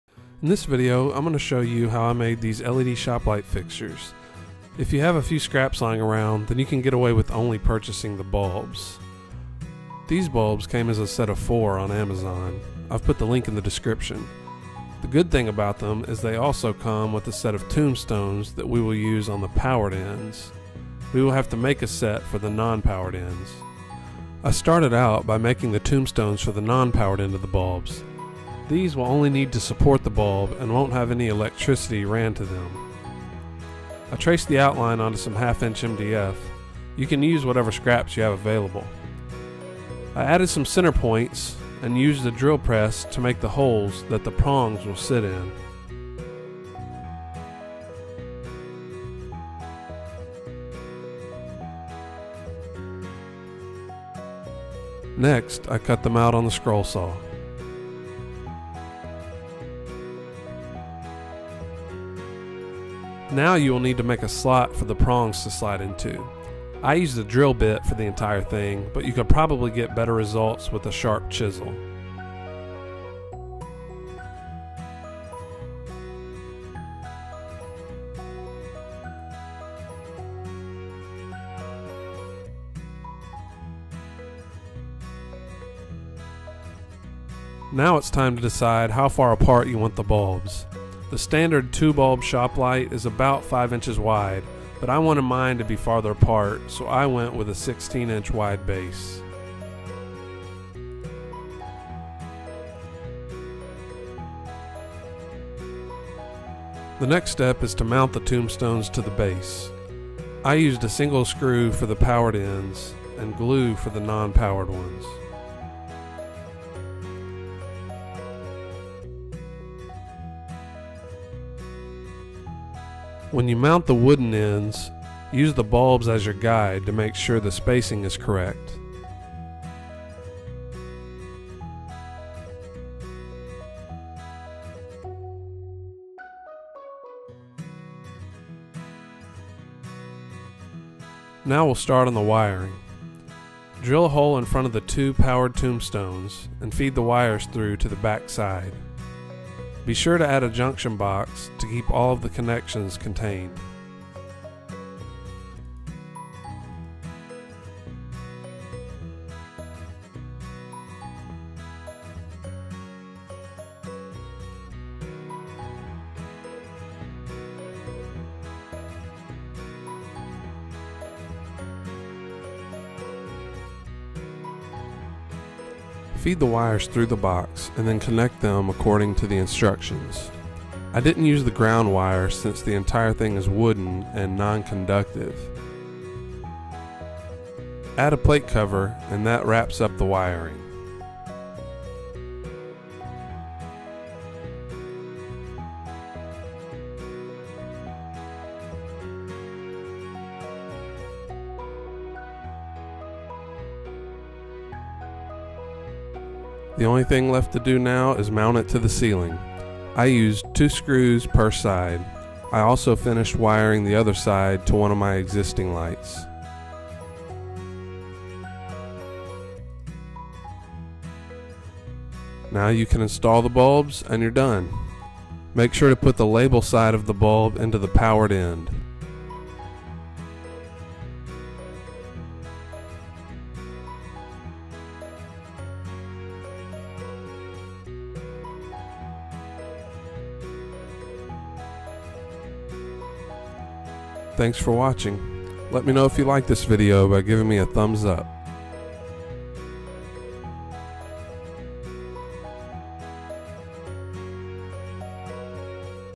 How to make led shop lights
I will show you how to make your own LED shop lights out of LED tubes sold online.

Bulbs used